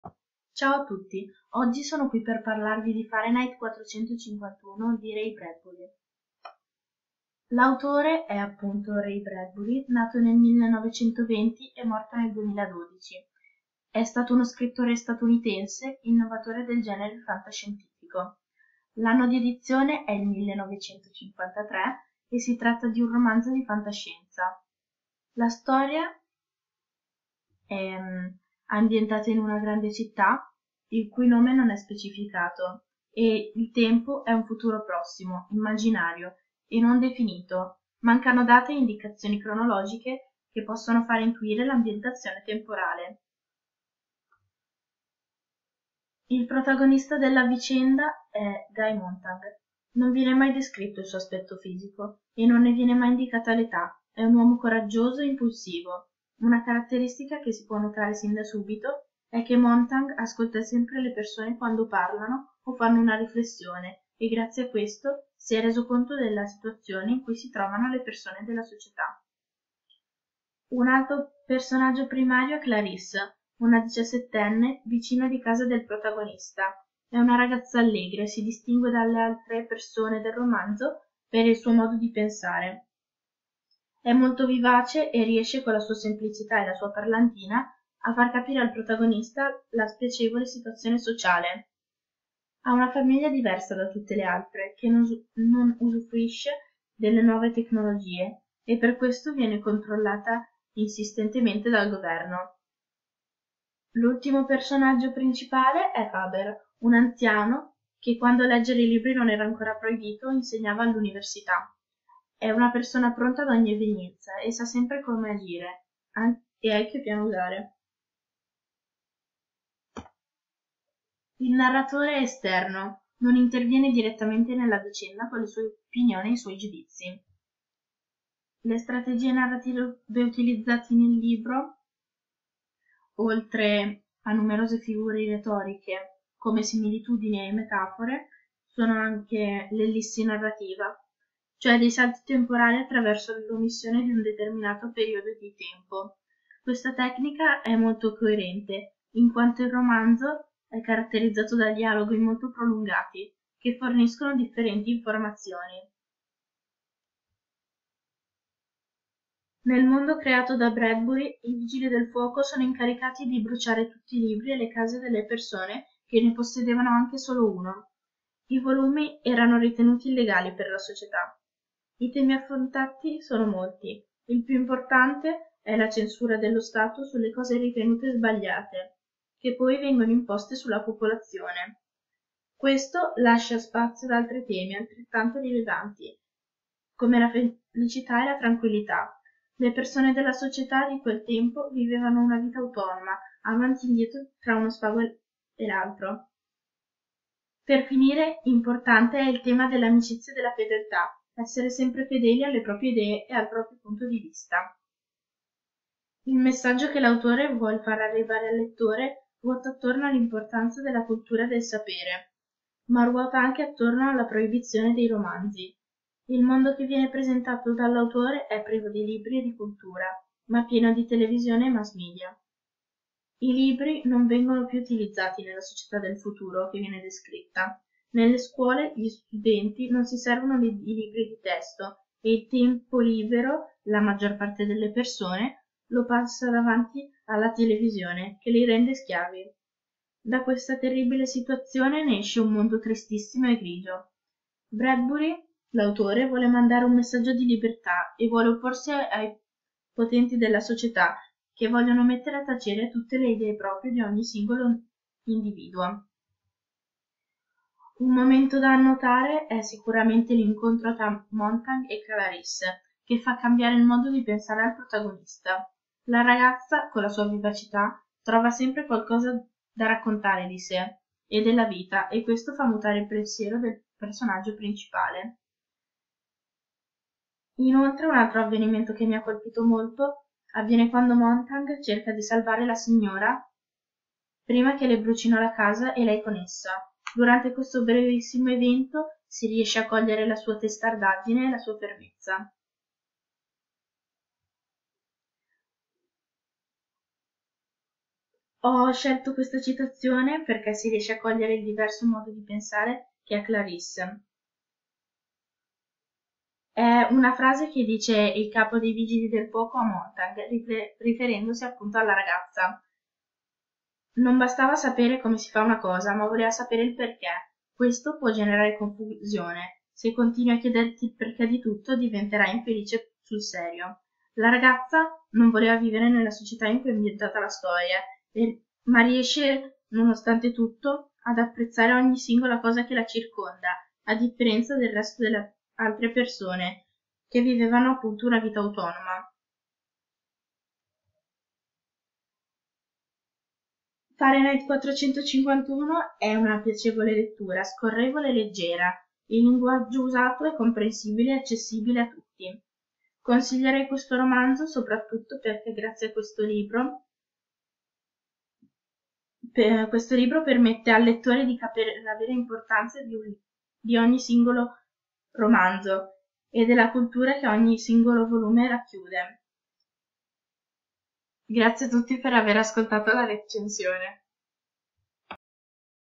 FAHRENHEIT 451 di Ray Bradbury
Videorecensione a cura di
Anna Ferrari
2C, Liceo S. A.
Ist. E. Fermi -Mantova